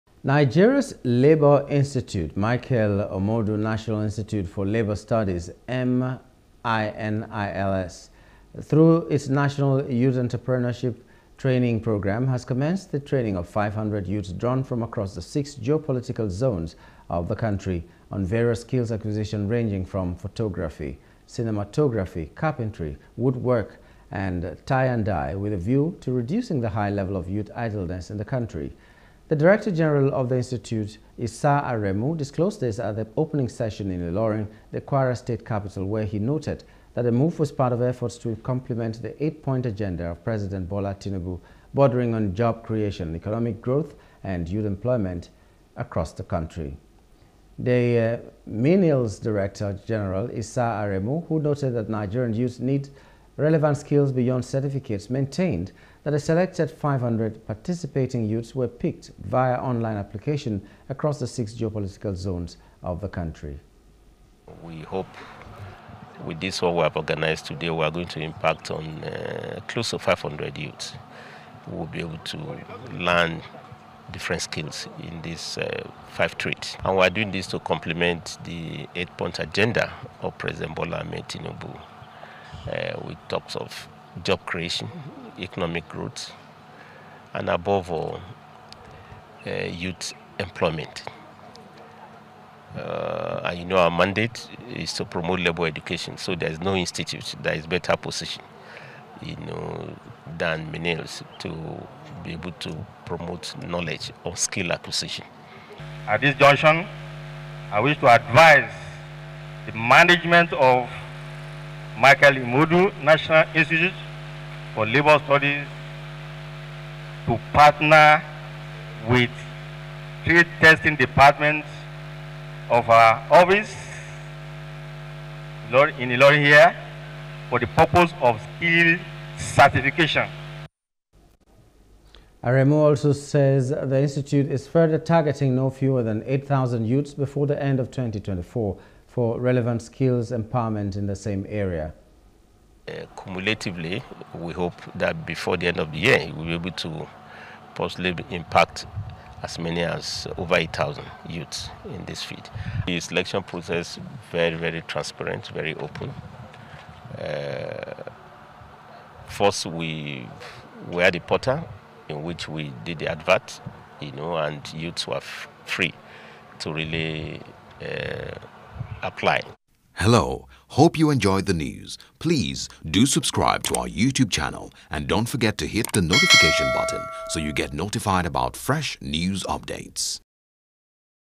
Entrepreneurship Devt: Minils Trains 500 Youths On Various Skills
Nigeria’s labor Institute, Michael Imoudu National Institute for Labour Studies (MINILS) through its National Youth Entrepreneurship Training Program has commenced the training of five hundred youths drawn from across the six geo-political zones of the country on various skills acquisition ranging from photography, cinematography, carpentry/woodwork, and tie and dye to reduce the high level of youth idleness in the country.